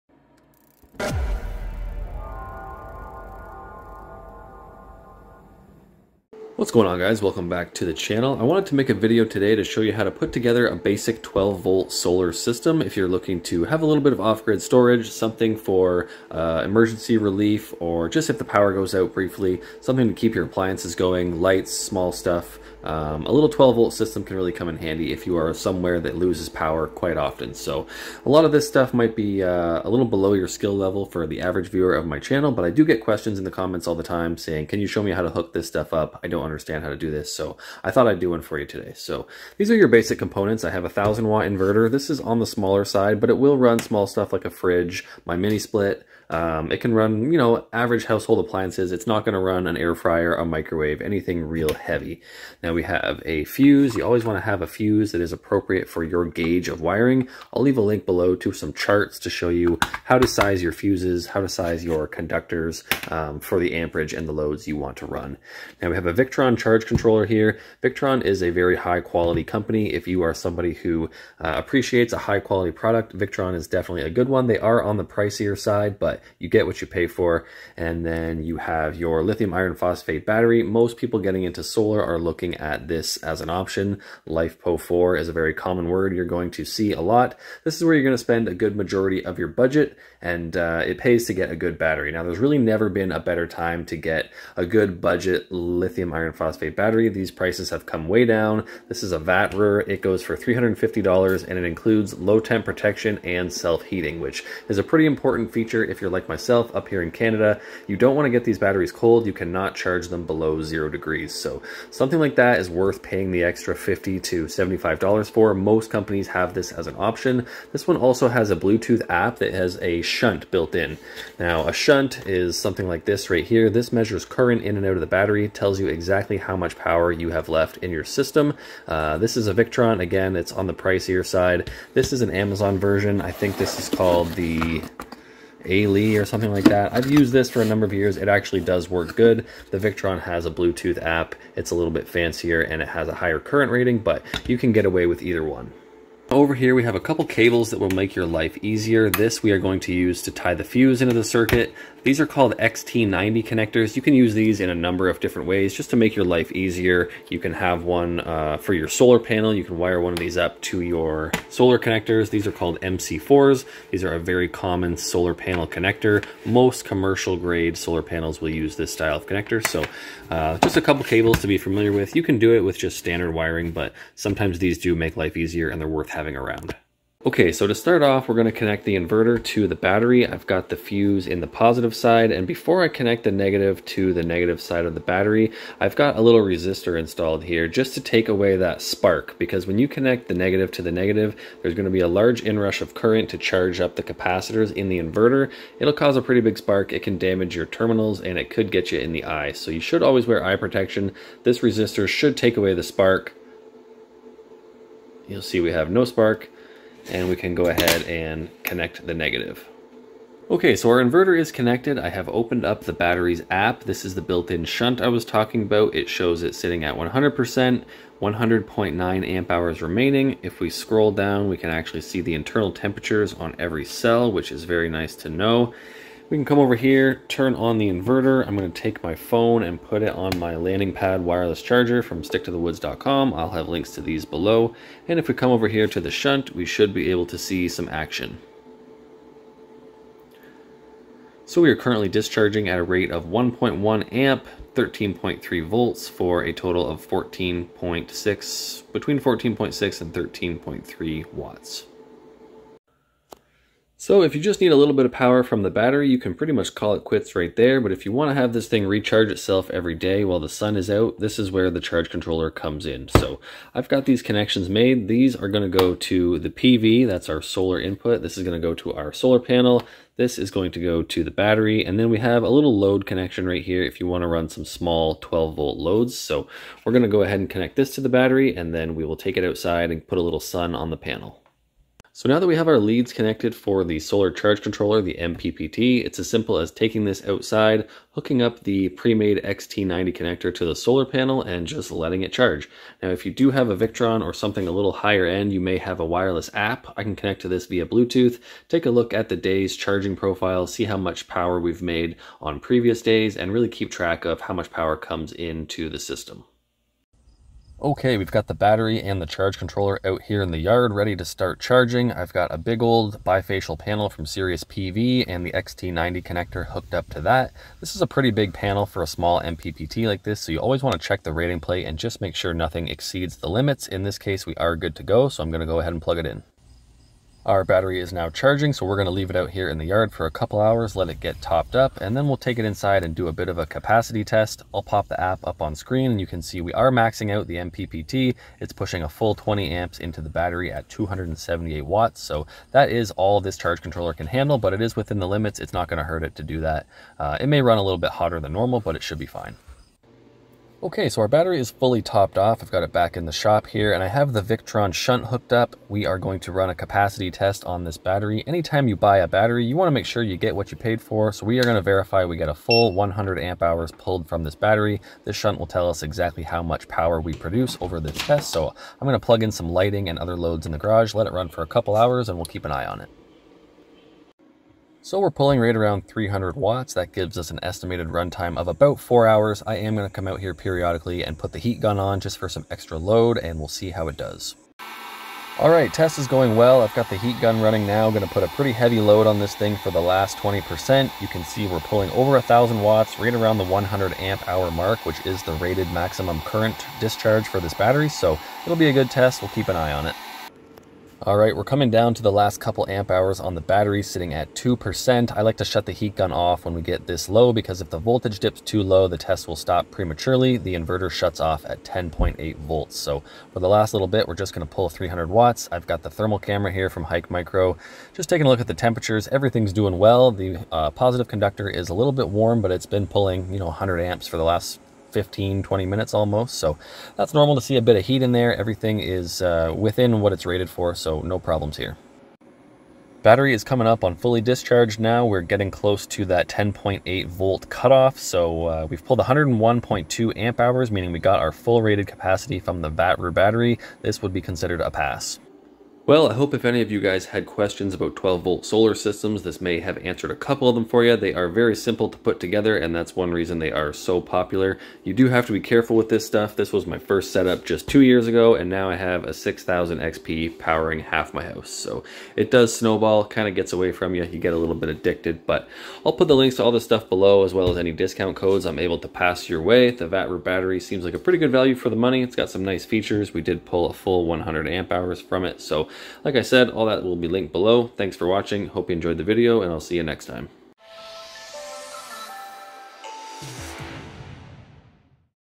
How To Build A DIY 12V Solar System *For Beginners* Vatrer 100AH
This is a straight forward guide on building a 12v solar system

Check out Sttw.ca and save 15% on your wireless charger purchase with code SmitHVAC15!